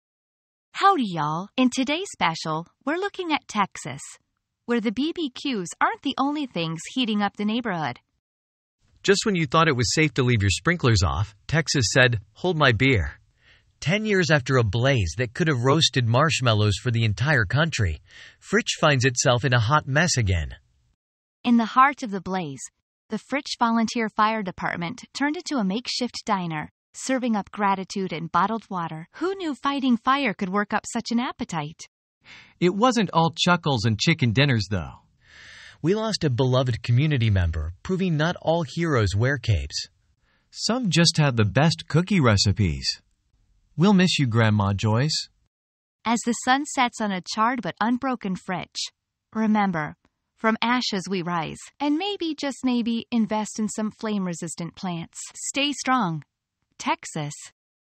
When Texas Fought Flames... Again! A Wildfire Recap with a Twist
Ten years after a devastating blaze, Texas faces the fury of flames once more. Dive into our unique take on this fiery tale, where humor meets homage.